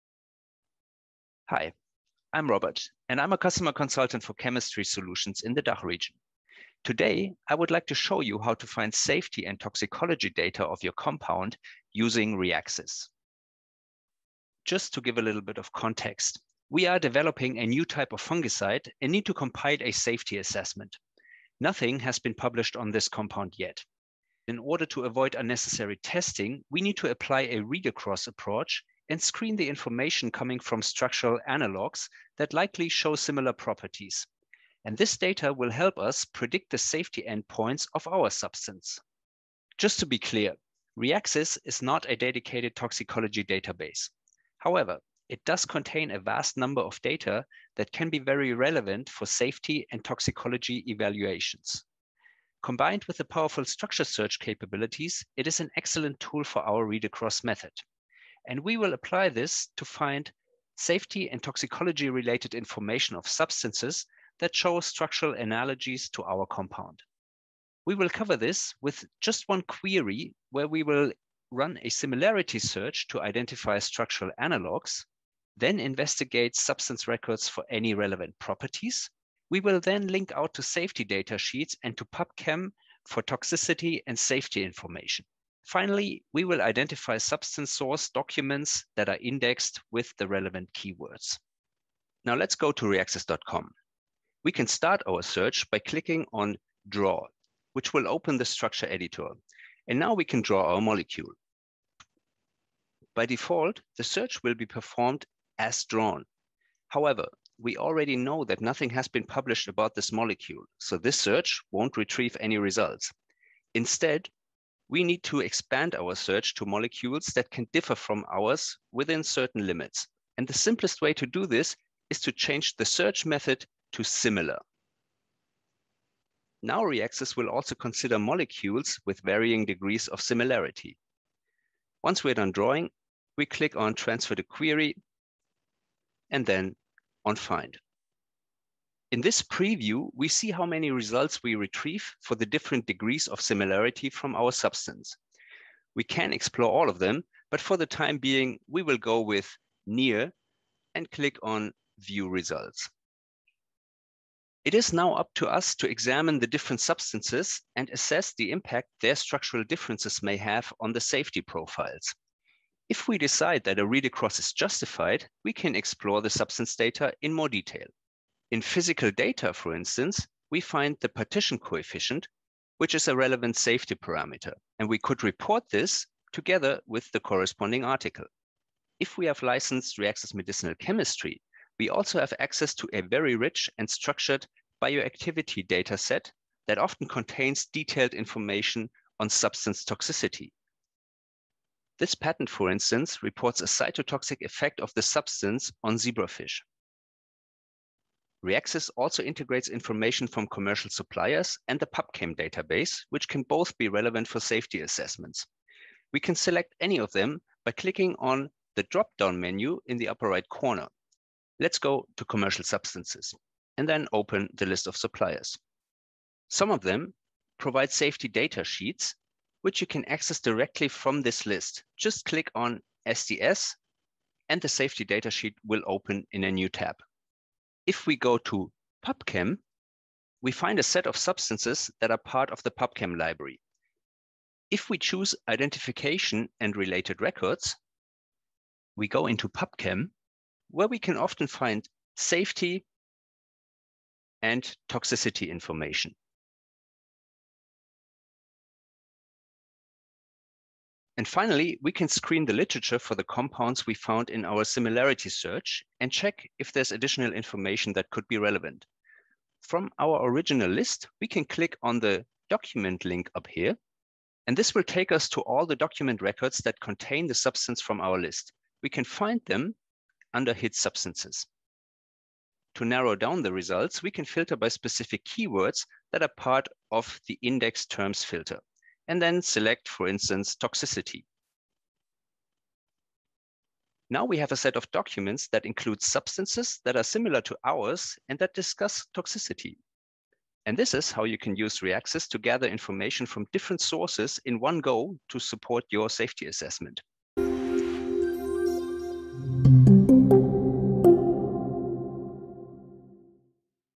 Reaxys - Find safety and toxicology data for your compound
Watch the new Reaxys tutorial on how to Find safety and toxicology data for your compound